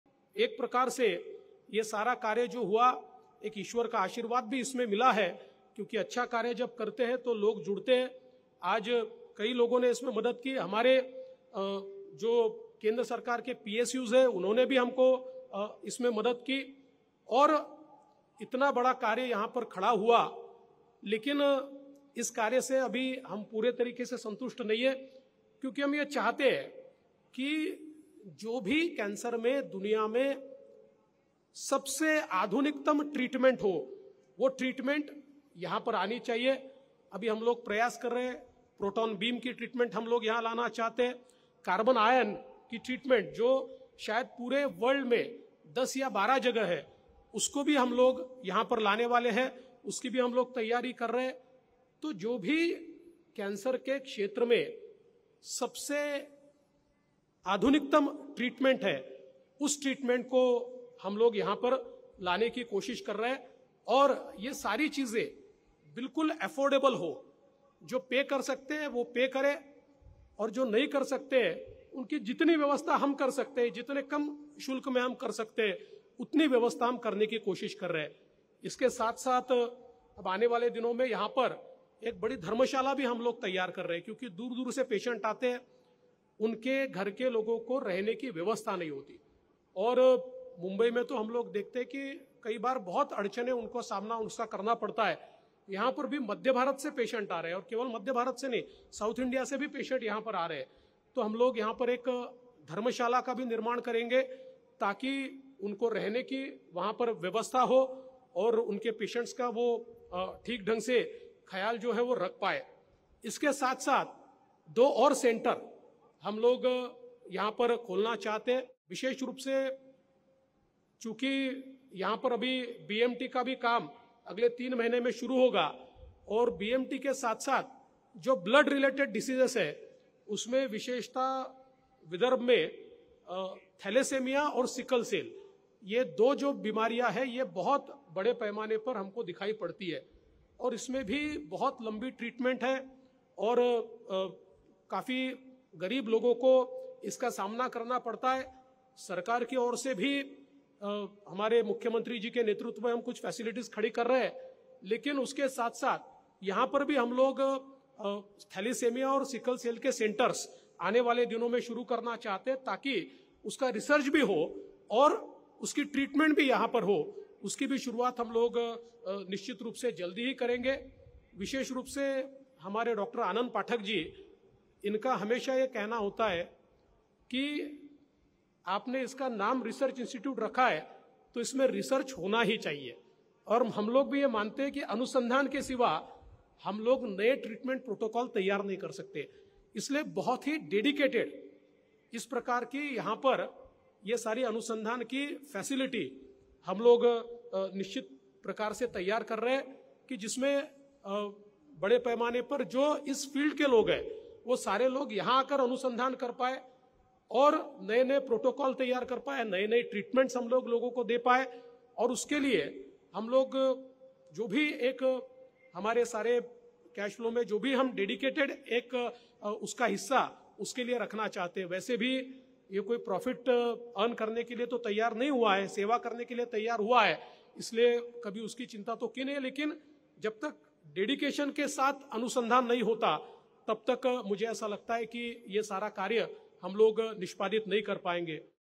नागपूरमध्ये कॅन्सरवर अत्याधुनिक उपचार होणार। नागपूर। DCM Devendra Fadnavis
नागपूरमध्ये कॅन्सरवर अत्याधुनिक उपचार होणार। नागपूर। DCM Devendra Fadnavis (Inauguration of National Cancer Institute, Nagpur | 27/04/2023)